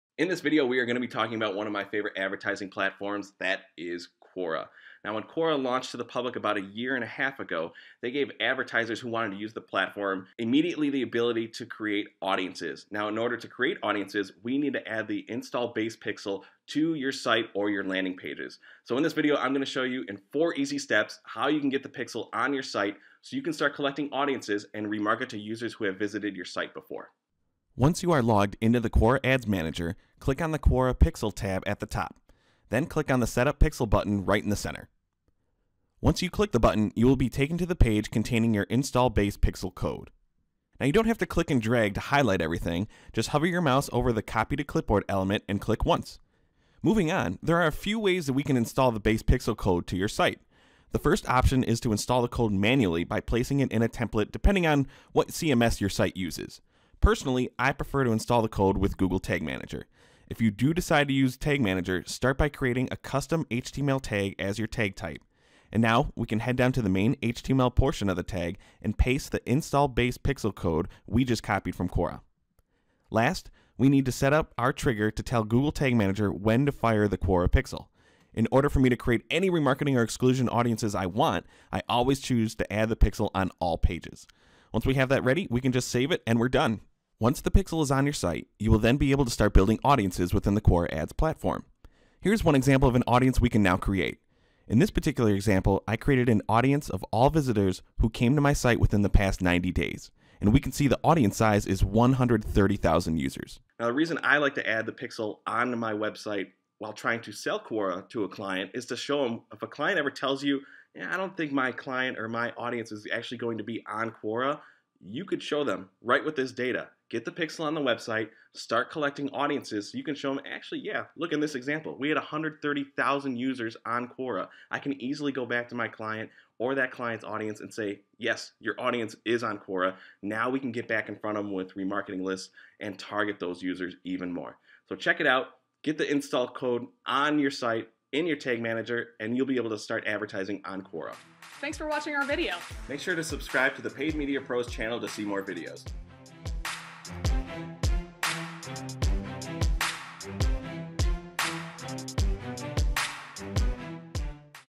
Quora Advertising Pixel Setup Instructions
Quora Advertising is now a must try channel for any PPC marketer. Quora now boasts more traffic than Pinterest and is showing continual signs of growth. One of the best features within the platform is the ability to start collecting audiences before you even have to give Quora Advertising your credit card information.

In this video, you can learn where to find your Quora Pixel and how easy it is to install on your site using Google Tag Manager.